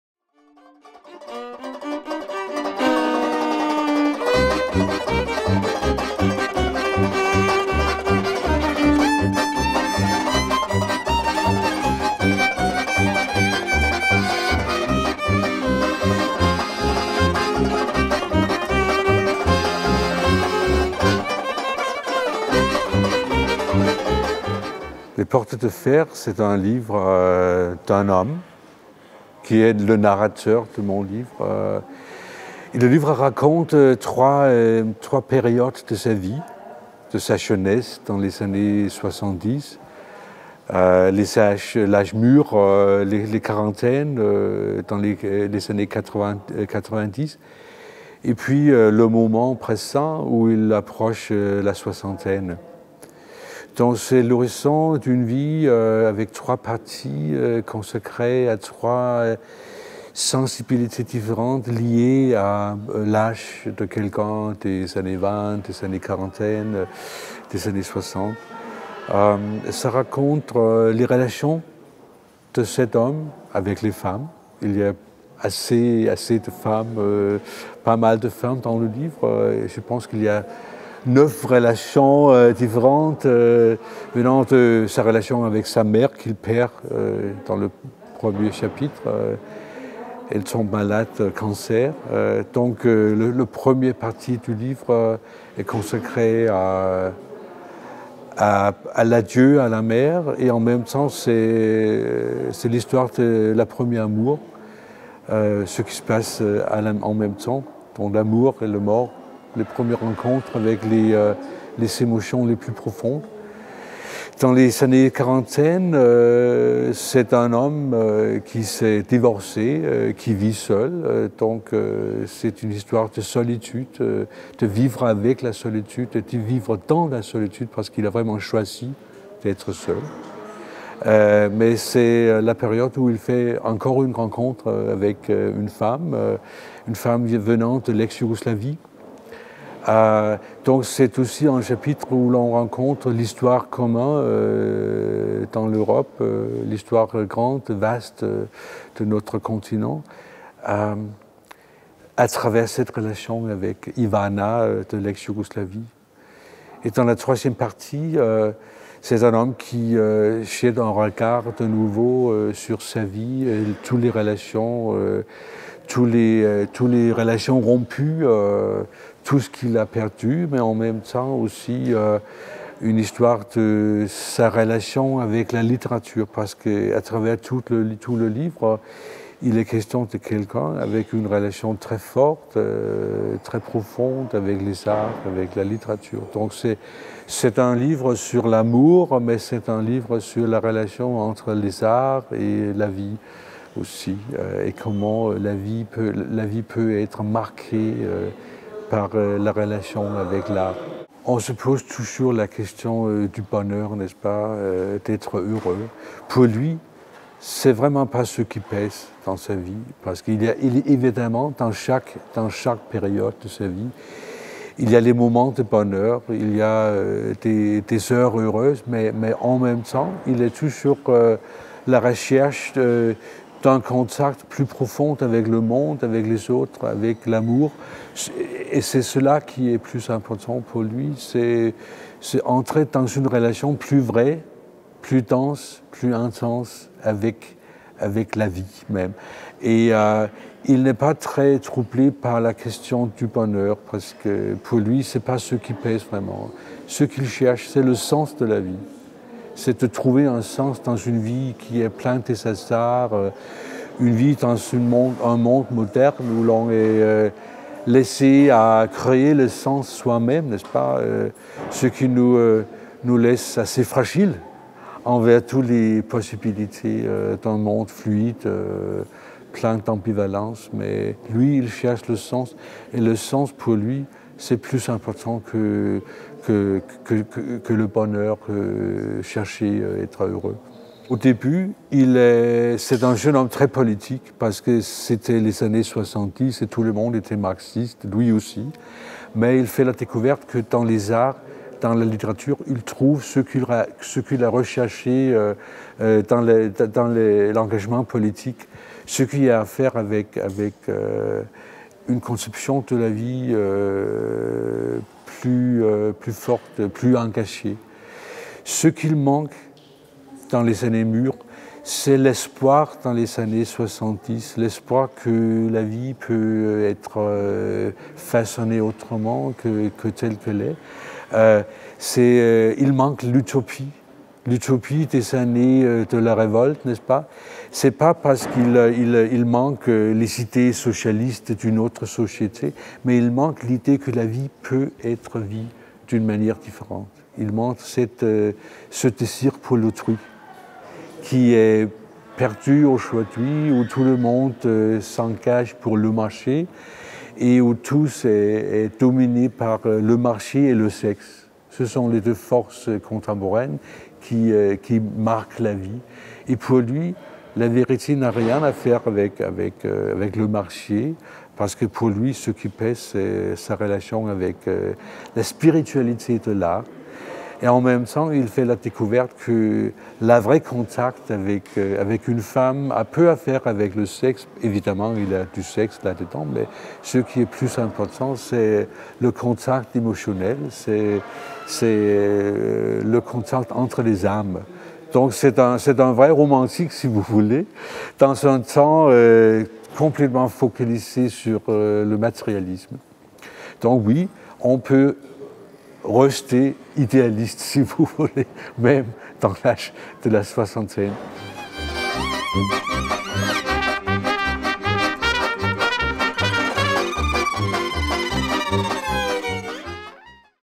Jens Christian Grondahl - Les portes de fer
Jens Christian Grondahl vous présente son ouvrage "Les portes de fer" aux éditions Gallimard à l'occasion de L'Escale du Livre 2016 à Bordeaux.